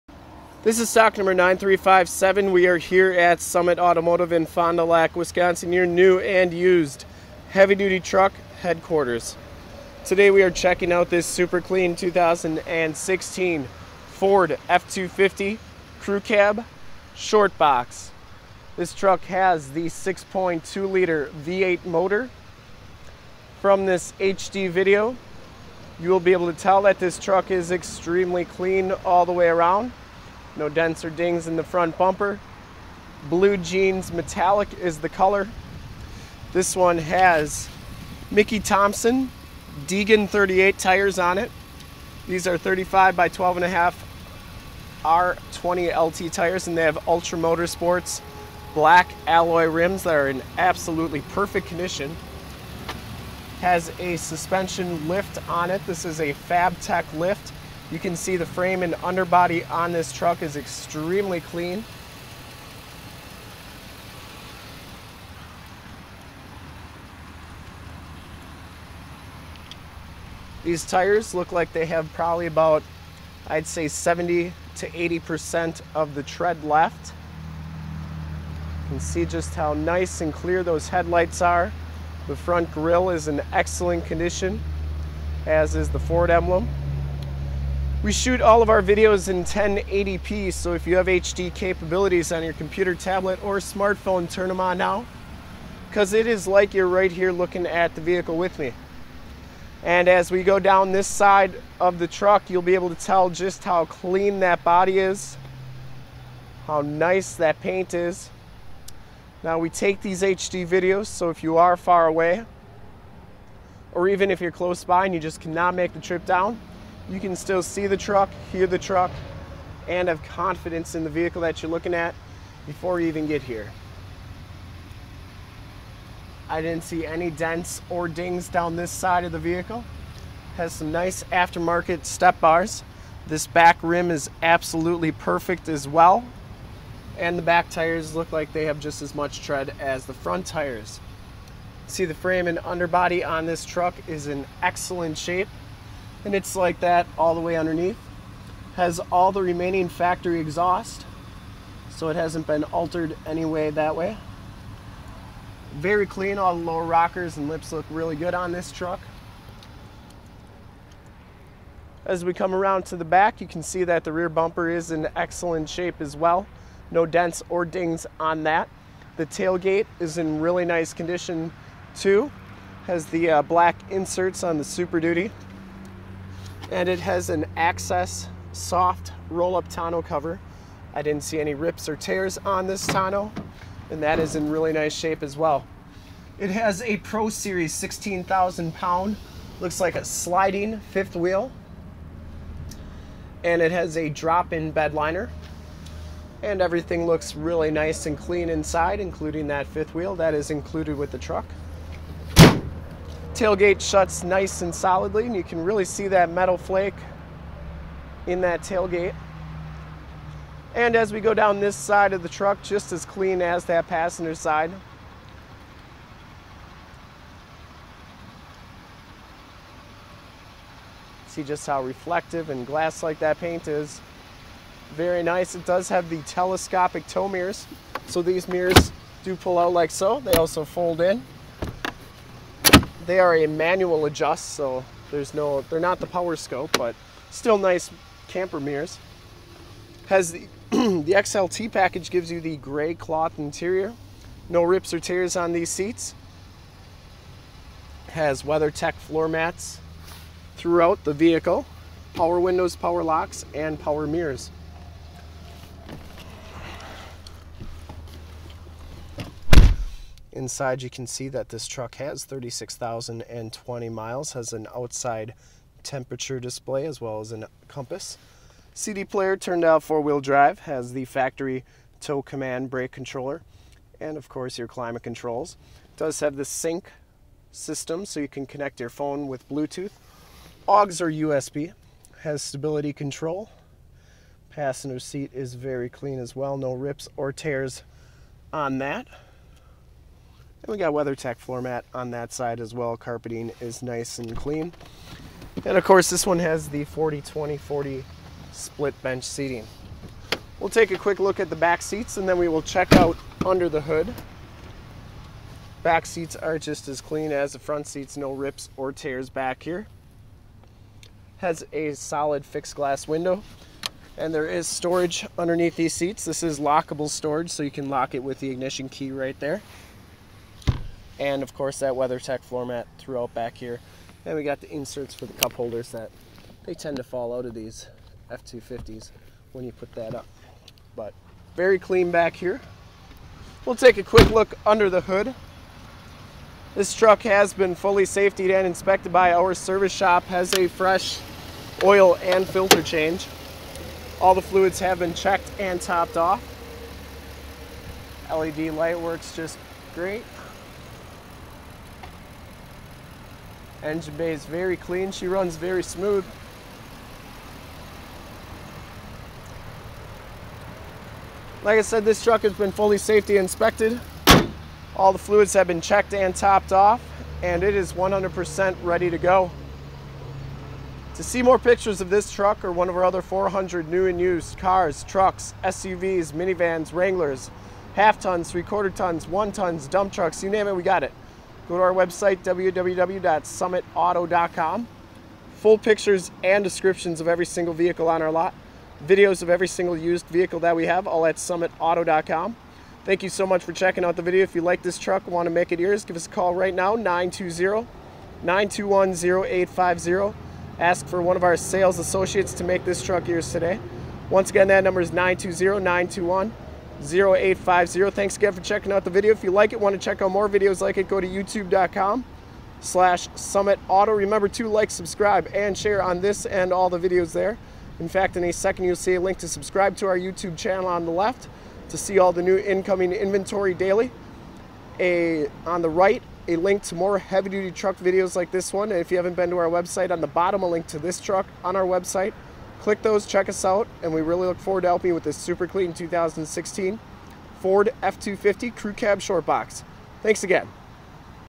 2016 USED FORD F250 CREW SHORT XLT LIFT BLUE JEANS METALLIC $36,999 SOLD! 9357
STOCK: 9357
PRICE: $36,999
MILES: 36,011
MAKE: FORD
MODEL: F250
VIN: 1FT7W2B67GEC84542

Clean TItle History! 6.2 Liter V8 SOHC Engine with Flex fuel capabilities E85, 385 Horsepower, Full Four Door Crew Cab, Short Box 6 1/2 Foot Shortbox, XLT Package, 6 Speed Automatic Transmission with Optional Manual Tap Shift, Turn Dial 4x4 Four Wheel Drive 4WD, Fabtech 6 Inch Suspension Lift Kit, Non Smoker, Gray Cloth Seats, 40/20/40 Split Front Bench Seating with Center Seat Hidden Storage Compartment, Access Soft Rollup Tonneau Cover, Full Towing Package with Receiver Trailer Hitch, Wiring and Transmission Cooler Tow Package, Pro Series 16K 5th Wheel Hitch Fifth Wheel, Fifth Wheel Bedrails 5th Wheel , Tow Command Factory Electric Brake Controller, Heated Telescopic Tow Power Mirrors with Built-in Directional Signals, Advancetrac with Roll Stability Control Traction Control, 3.73 Gears, Mickey Thompson Deegan 35x12.50 R20 LT Tires, Ultra Black 20 Inch Rims Premium Wheels, Painted Alloy Rims Premium Wheels, Four Wheel Disc Brakes, Aftermarket Chromed Stepbars, Drop-in Bedliner, Bedrail Covers, Raise Assist Tailgate, Vent Shades, Locking Tailgate, Chrome Trimmed Grill, AM / FM Radio Tuner, Sirius/XM Satellite Radio Capabilities Sirius / XM, CD Player, Microsoft SYNC System with Bluetooth, Hands-Free Audio System Blue Tooth, Auxiliary MP3 Jack Portable Audio Connection, USB Jack Portable Audio Connection, Keyless Entry System, Adjustable Height Seatbelts, Driver and Passenger Front Air Bags, L.A.T.C.H. Child Safety System, Side Curtain Air Bags SRS Safety Restraint System, Multi-Function Steering Wheel Controls, Compass, Outside Temperature Display and Mileage Display, Weathertech Floormats, Storage Compartment Under Rear Seats, Air Conditioning AC, Cruise Control, Power Locks, Power Windows, Tilt/Telescope Steering Wheel, 5 Year / 60,000 Mile Remaining Powertrain Factory Warranty, Whichever comes first, Blue,

STOCK: 9357
PRICE: $36,999
MILES: 36,011
MAKE: FORD
MODEL: F250
VIN: 1FT7W2B67GEC84542